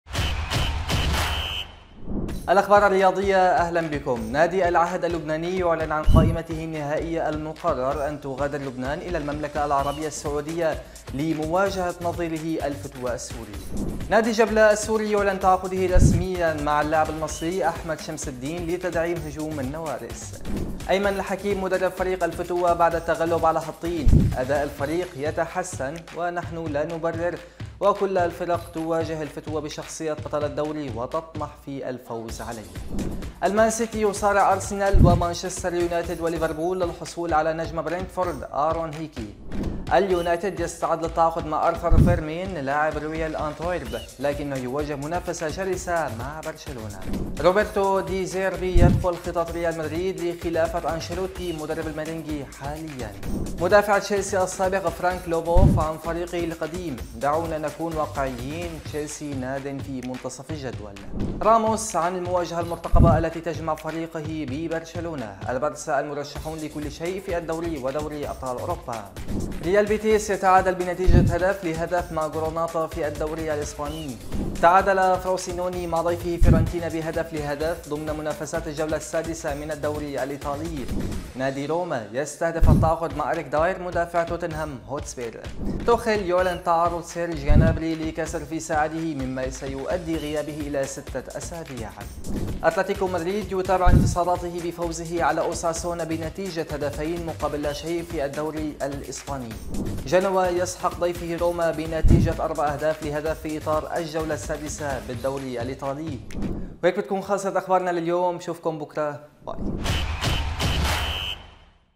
ريال مدريد يسجل أسوء رصيد تهديفي في التاريخ . ومواجهة بين برشلونة ومانشستر يونايتد على آرثر فيرميرين
المان سيتي يصارع آرسنال ومانشستر يونايتد وليفربول، للحصول على نجم برينتفورد، آرون هيكي.
-مانشستر يونايتد يستعد للتعاقد مع آرثر فيرميرين، لاعب رويال أنتويرب لكنه يواجه منافسة شرسة من برشلونة
-روبرتو دي زيربي يدخل خطط ريال مدريد لخلافة أنشيلوتي مدرب المرنجي حالياً
-مدافع تشيلسي السابق فرانك لوبوف عن فريقه القديم: دعونا نكون واقعيين تشيلسي نادٍ في منتصف الجدول
-راموس عن المواجهة المرتقبة التي تجمع فريقه ببرشلونة: البرسا المرشحون لكل شيء في الدوري ودوري الأبطال
-ريال بيتيس يتعادل بنتيجة (1-1) مع غرناطة في الدوري الإسباني